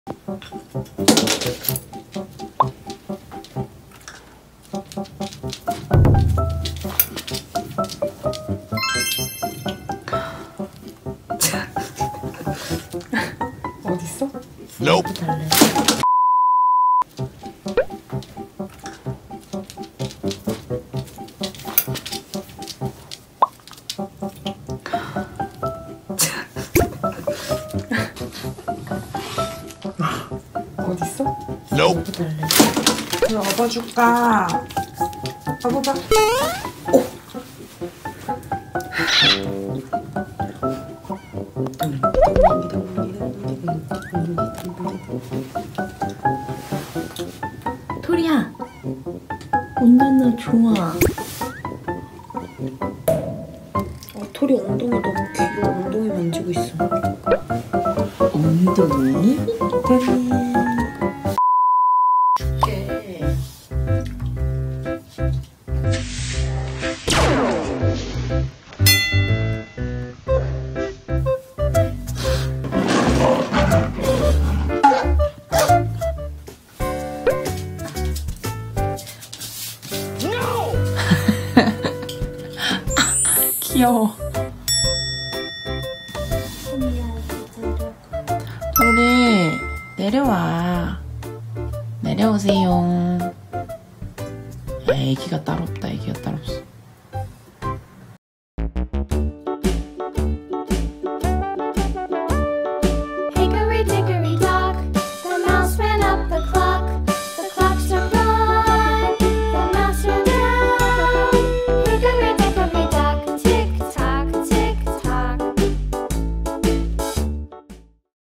업어달라고 최애간식 조공하는 강아지
왕년에 좀 뛰어본 토리입니다 ㅋㅋㅋㅋ 어쩌다가 이지경까지 됐는지는 알 수 없어요 ... 가르치지도 않은것을 깨우치다니 ... 너가 정녕 강아지가 맞느냐?..

*핸드폰 촬영이라 화질이 좋지 못한점 양해부탁드립니다 흑흑 저를 때리세요

💕토리야 사랑해💕

갱얼쥐 : 푸들
이름: 도토리
나이 :4쨜
성별 : 남 (내시임)
몸무게 : 8kg

많이 많이 봐주세요!
사랑스러운 애기니 악플은 삼가해주시면 감사하겠습니다 ♡